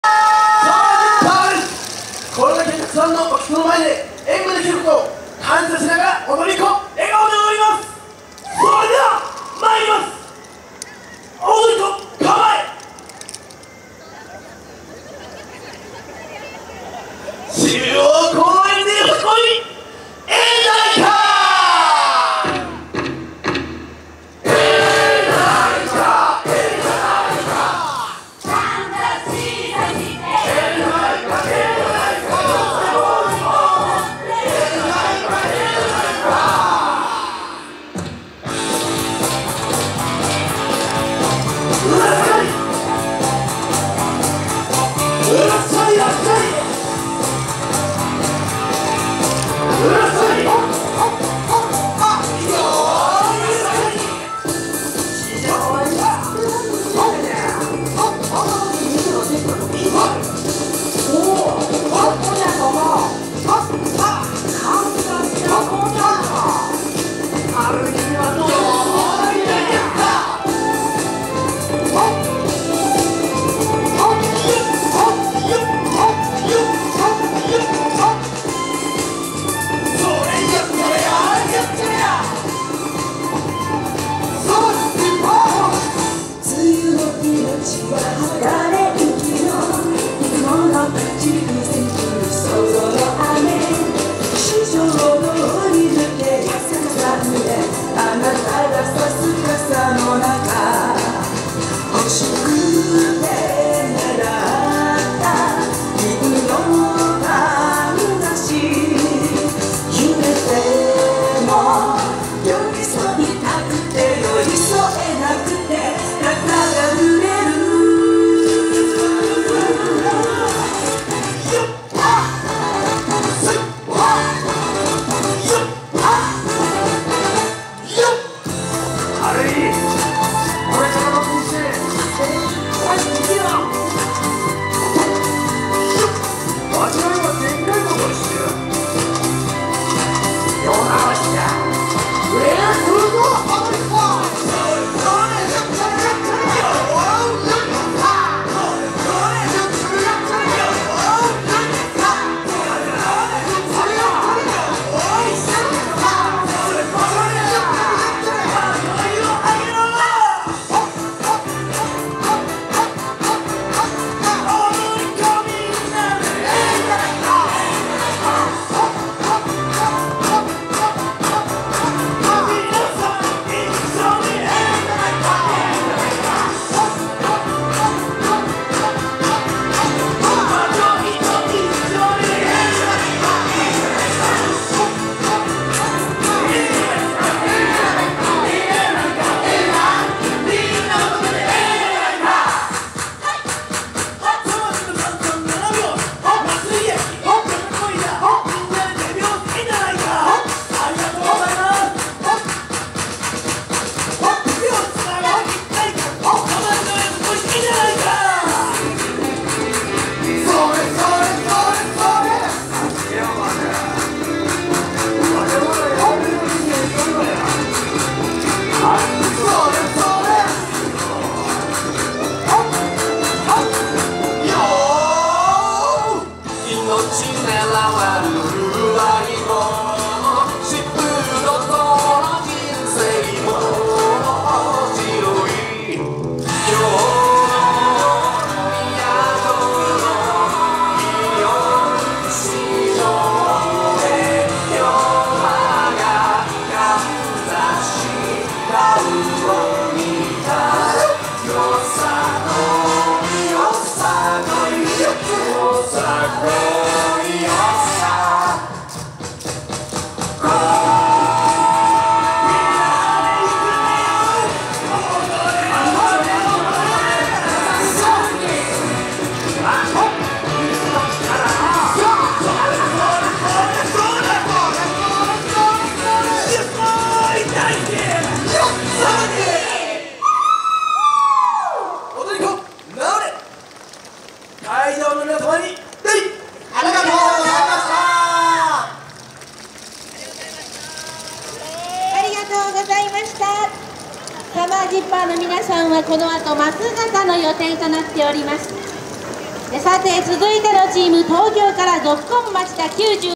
[4K] Ｓｕｍｍｅｒ Ｚｉｐｐｅｒ 高知よさこい祭り 2019 本祭2日目 (中央)
2019年8月11日(日)
第66回よさこい祭り　本祭2日目